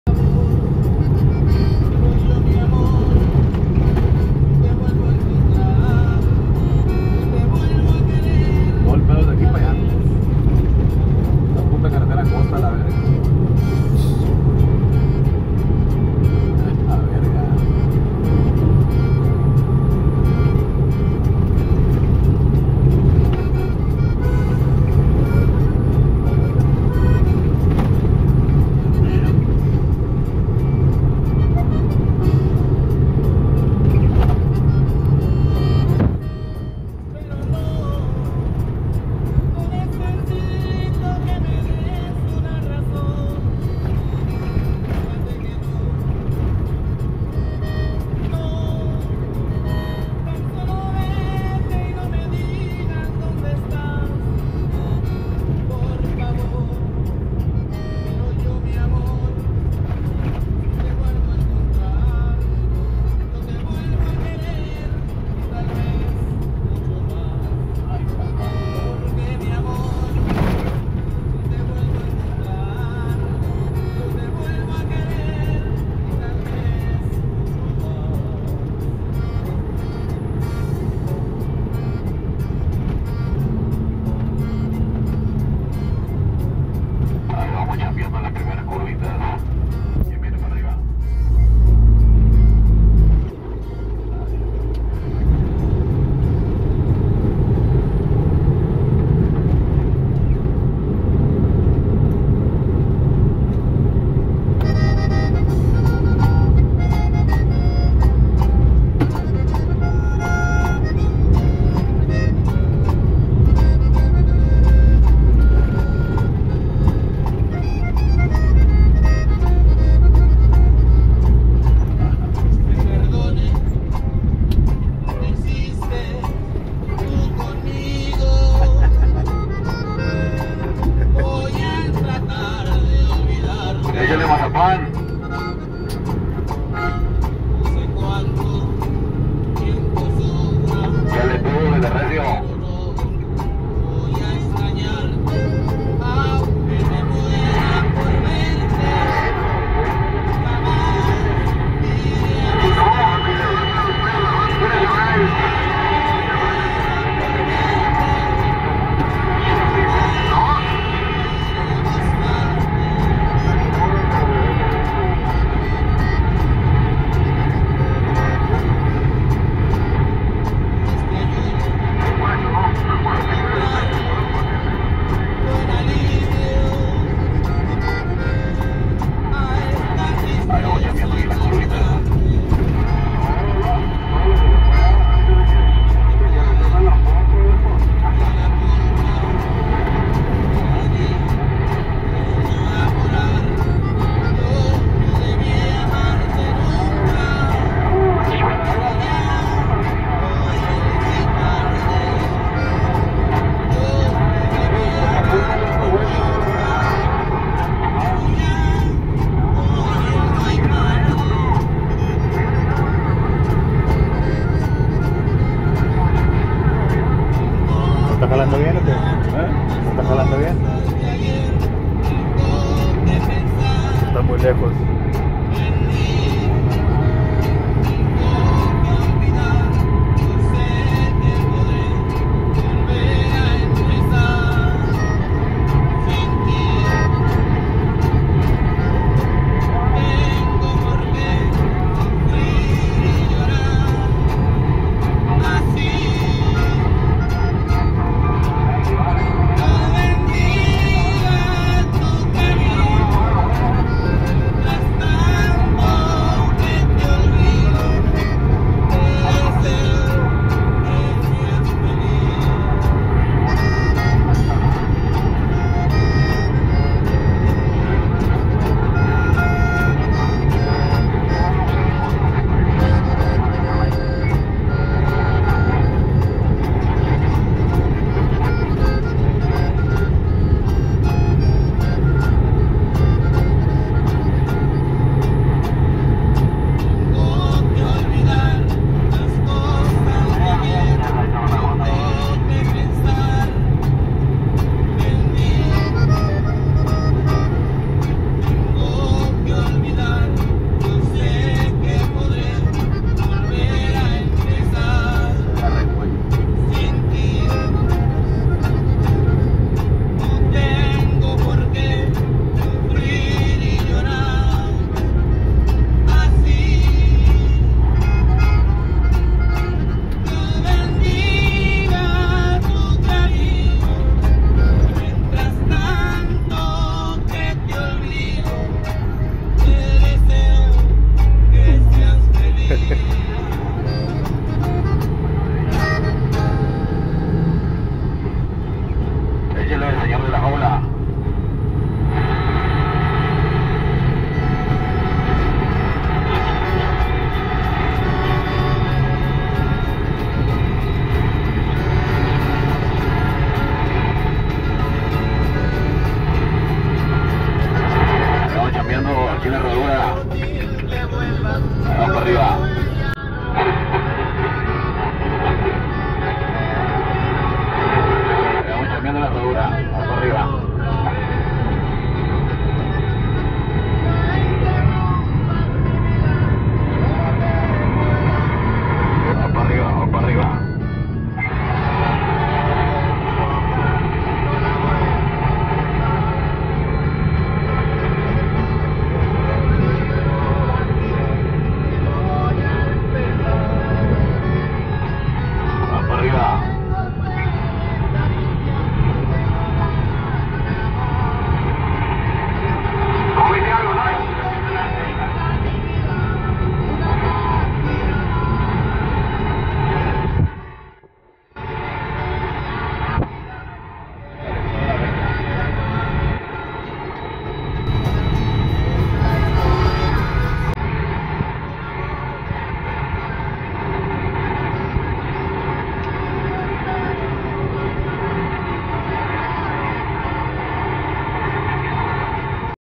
Subiendo y bajando sierra de san luis (la herradura)
Subiendo y bajando sierra de san luis (la herradura) Janos Chihuahua- Agua Prieta Sonora